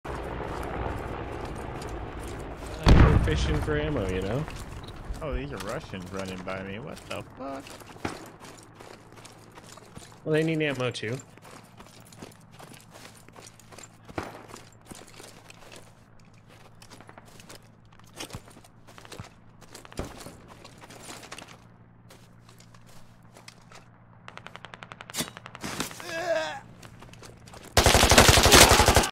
Confused Russian - ICO Update
Encountered a few Russians that didn't seem to realize I wasn't on their team